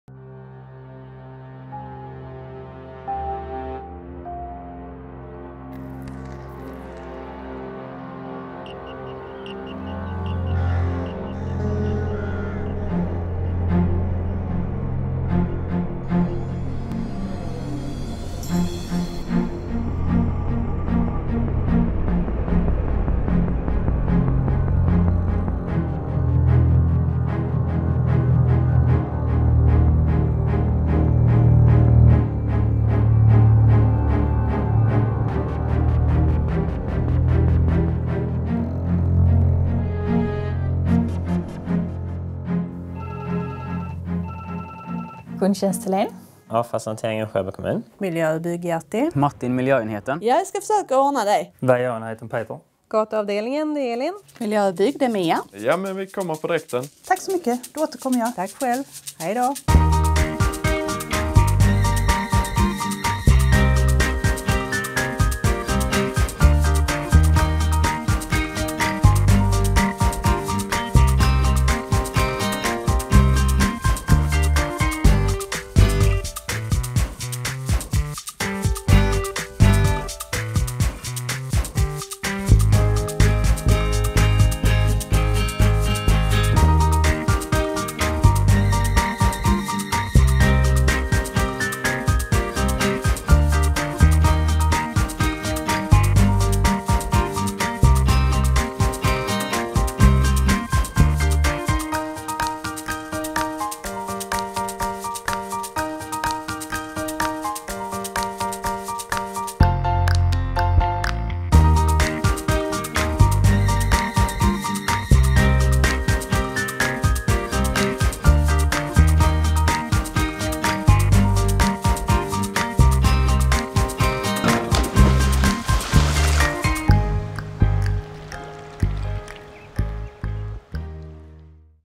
Sjöbo kommun, teknisk service
Det här är en film som visar en del av den teknisk servicen i Sjöbo kommun, bl a VA, avfallshantering, kundtjänst.